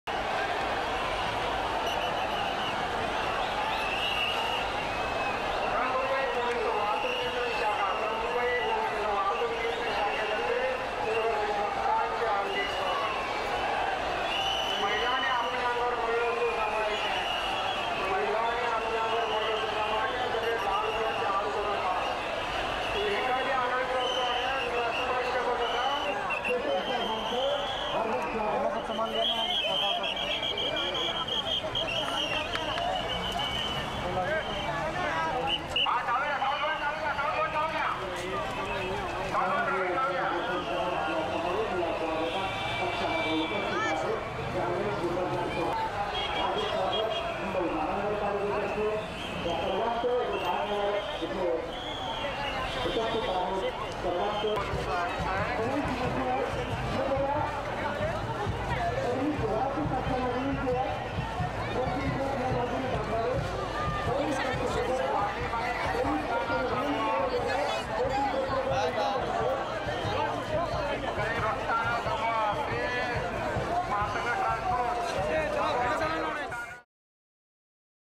Taking the Ganapati Idol for immersion: Ganpati visarjan Mumbai
As per the science of Spirituality, the divinity generated in the Idol consecrated on the day of Ganesh Chaturthi lasts only for a day. That is why the Idol should be immersed in water that very day or the next day, after performing ritualistic worship.
source: HJS